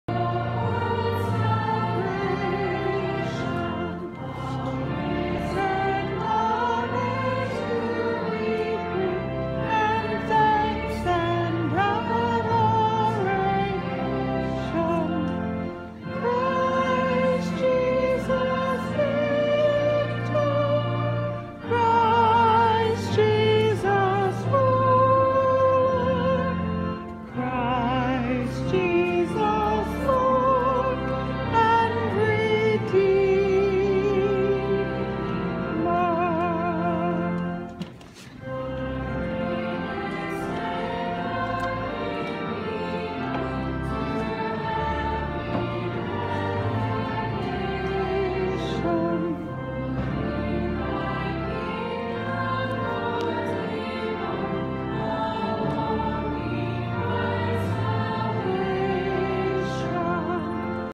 Confirmation and First Holy Communion
May 21, 2017, Christ The King Chapel, with Bishop Tissier, SSPX.